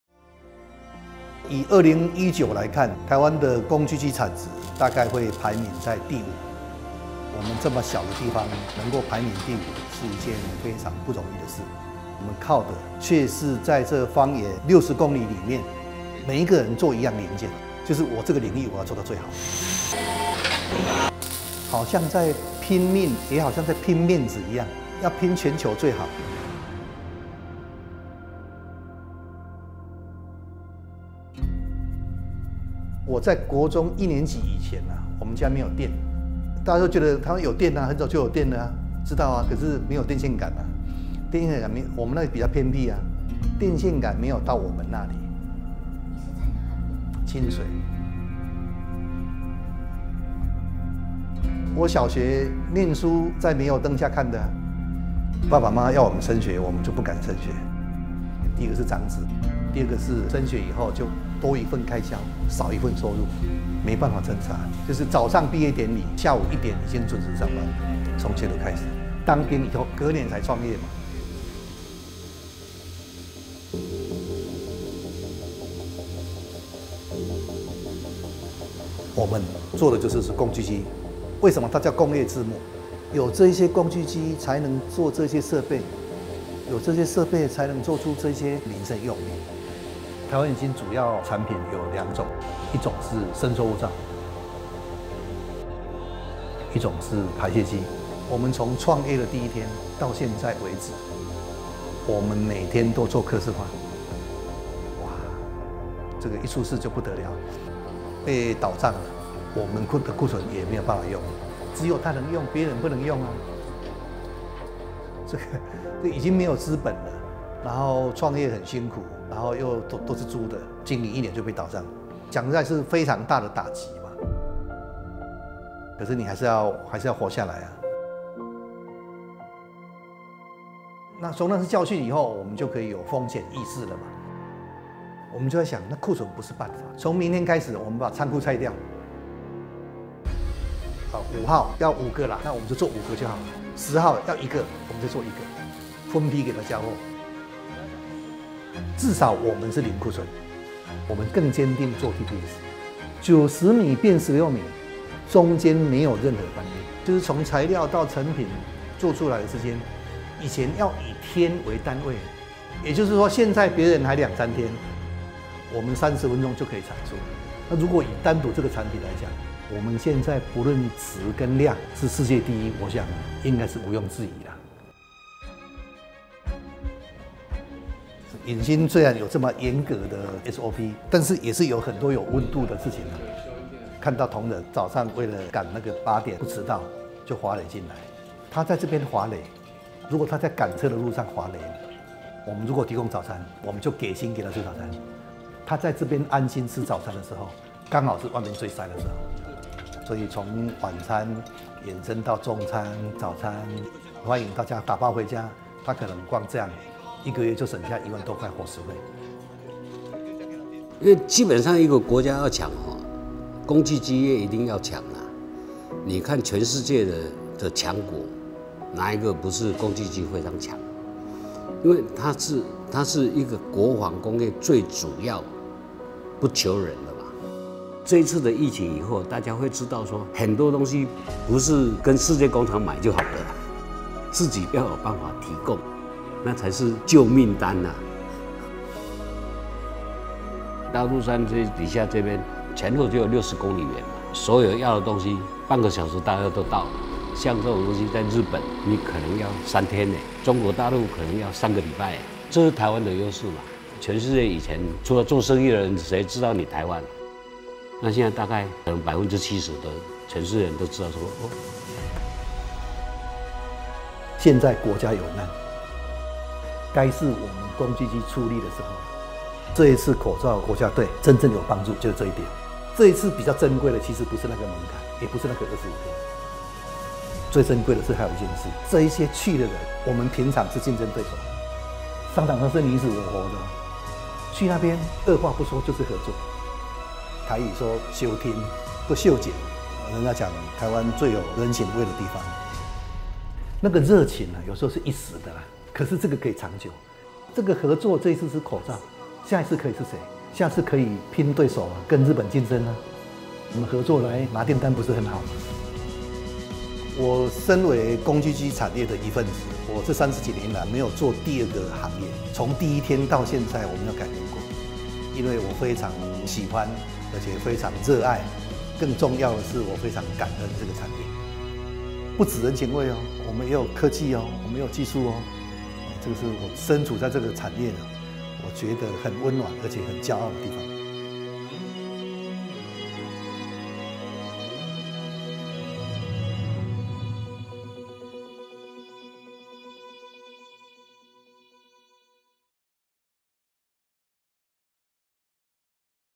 一起拚下去》口罩國家隊神救援！大肚山上的全球工具機伸縮護罩之王｜小鎮台霸系列
台灣工具機產業在世界上名列前茅，而這一次，他們組成口罩機國家隊，讓台灣在防疫上稱冠。

這個產業是一個相互幫襯的產業：八成在方圓六十公里內、每家公司都不大，把一項零件做到極精極深。

如台灣引興是全球工具機伸縮護罩之王，董事長王慶華靠著精實管理，將產線縮短了八成、將原本數天的製程縮短到三十分鐘。也靠著跟群聚的緊密連結，創造零庫存奇蹟。

從前我們介紹的小鎮台霸，是隱身在鄉間的冠軍；這次想說的是群聚共榮，所創造出的台霸。

每一場光榮戰役裡，除了將軍之外，更珍貴的是各司其職、各展所長的戰士。經過這次疫情後，台灣人更懂：每個挺身而出的戰士，都是真正的英雄。

■延伸閱讀：台灣引興》大甲溪佃農之子　流水哲學打造全球第一